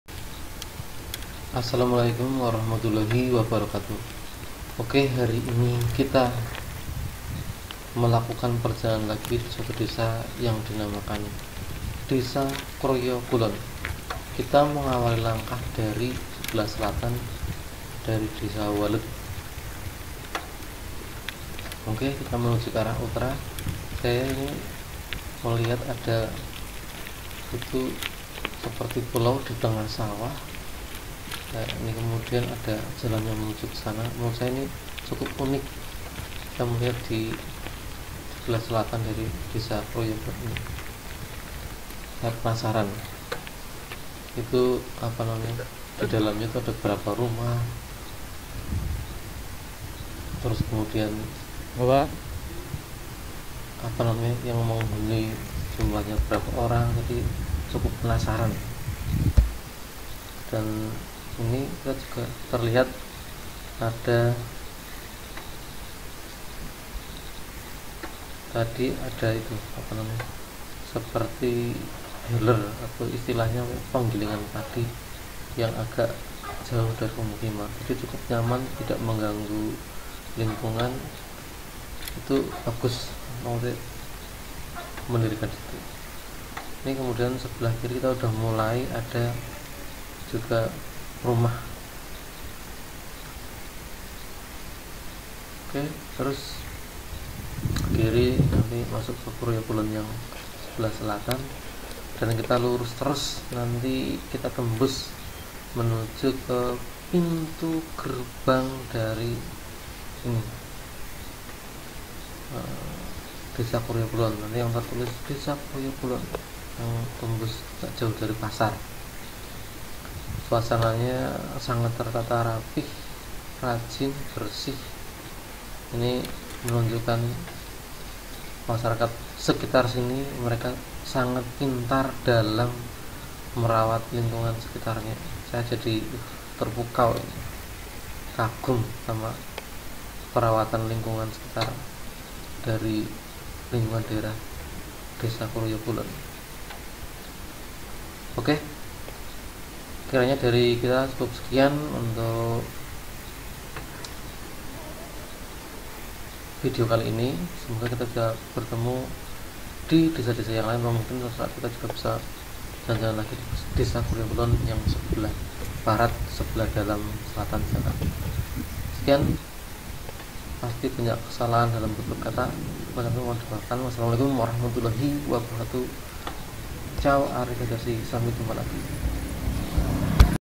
DESA KROYO KULON KEMIRI PURWOREJO BAGIAN 2
Desa ini berbatasan dengan beberapa Desa diantaranya ;
Sebelah Utara Desa Kroyo Lor
Sebelah Selatan Desa Gesikan dan Waled
Sebelah Barat Desa Ngampel
Sebelah Timur Desa Rejosari dan Desa Kroyo Lor